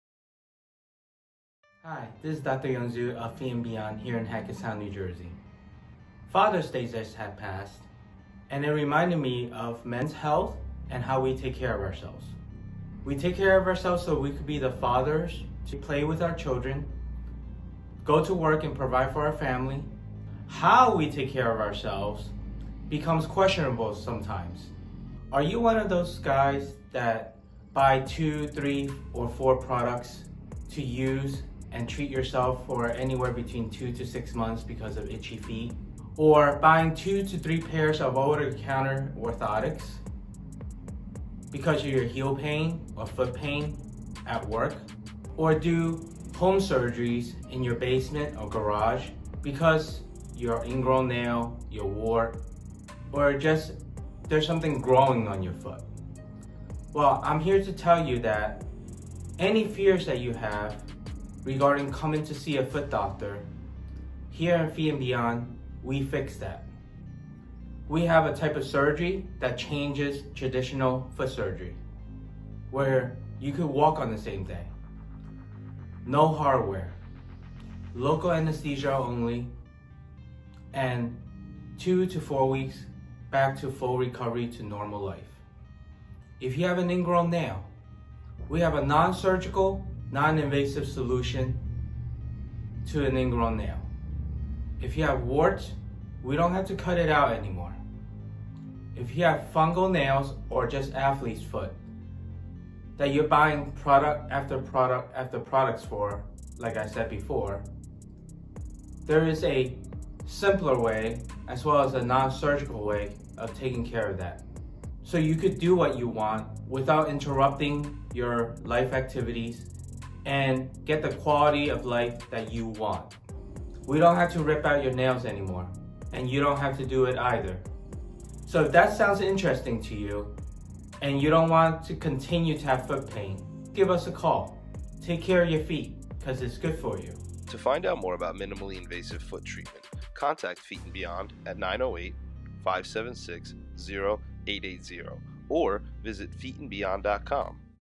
Men's Health Awareness
Take care of your feet...
Becasue it's good for you!

A strong message from Dr. Zhu for all the men out there.

Your health journey just got a whole lot more connected.